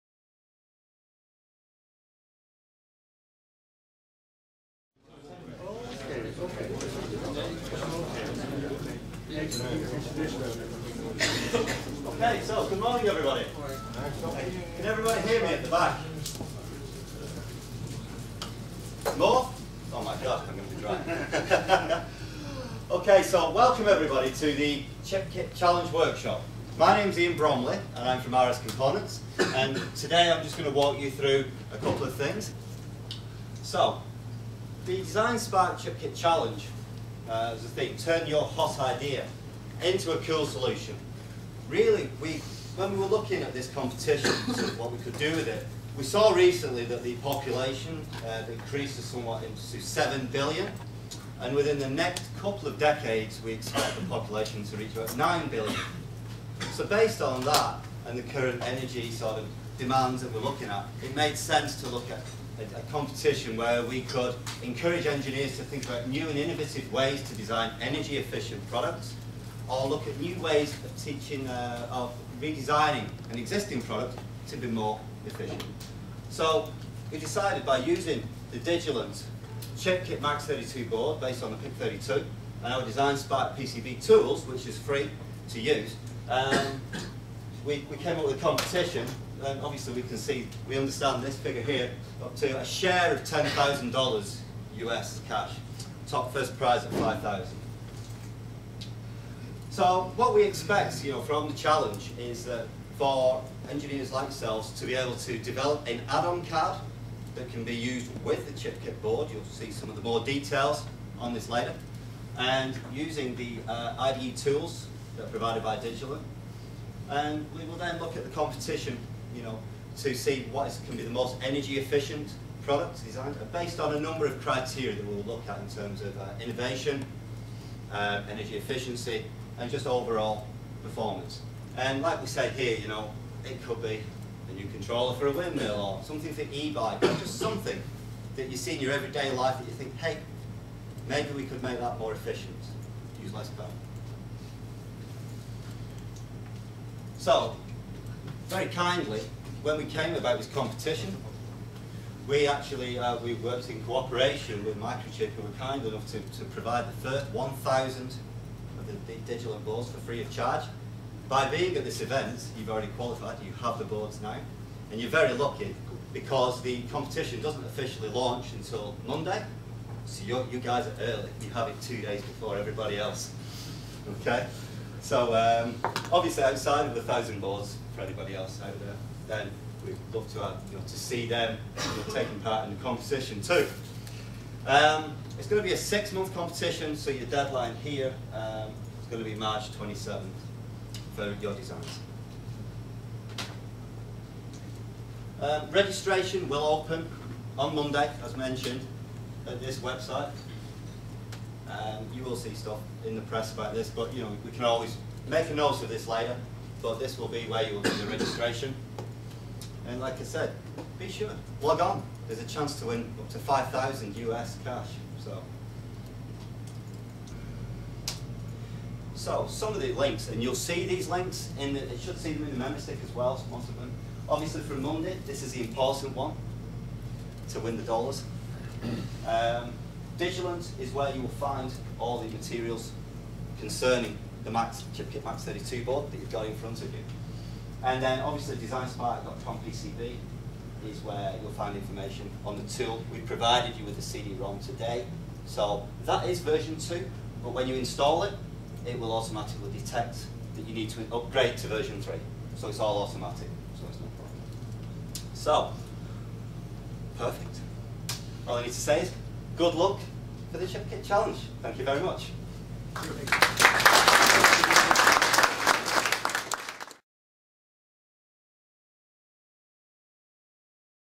Elektor - DesignSpark chipKIT™ Challenge
TURN A HOT IDEA INTO A COOL SOLUTION
Challenge your talent against other engineers worldwide to produce an open source energy-efficient design solution using the Free DesignSpark PCB software and the chipKIT™ development board.
Achieve the most energy-efficient design and you could win a share of $10,000 cash!
Plus, keep the DesignSpark community regularly informed through posts on the DesignSpark Project Pages and your updates will make you eligible for Community Choice Awards and random prize drawings.